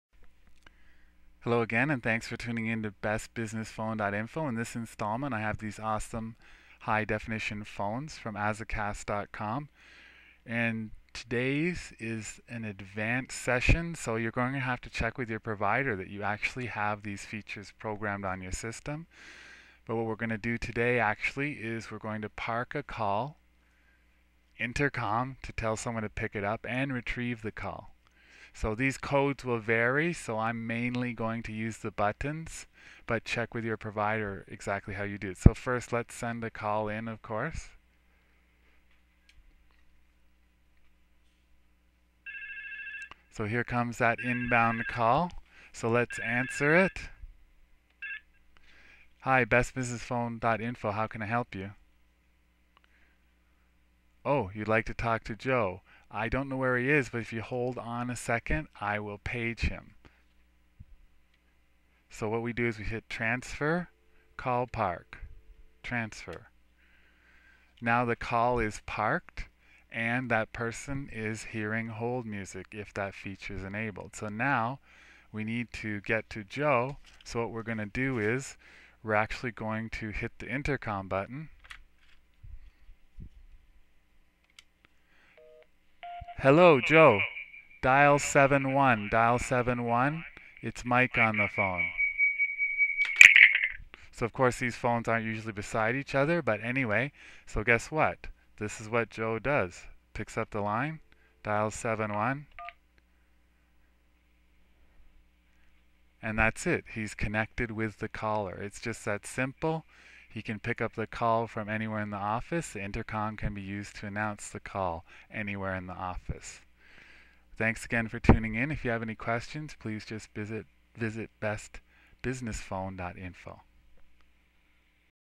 C1 HD High Def Phone -- Call Park, Intercom / Paging and Call Pickup
Answer the call in the normal way ( handset, Speaker button, or Flashing green line button)
Press TRANsfer, CALL PARK, TRANsfer to park the call, the system will annouce the PICKUP code
Press INTERCOM button ( or code), to PAGE your co-worker, " Joe Dial 71, its Mike from ABC Co."
Joe, go to any phone on the system
Press 71 ( or other specified code) to be connected with Mike
Thanks,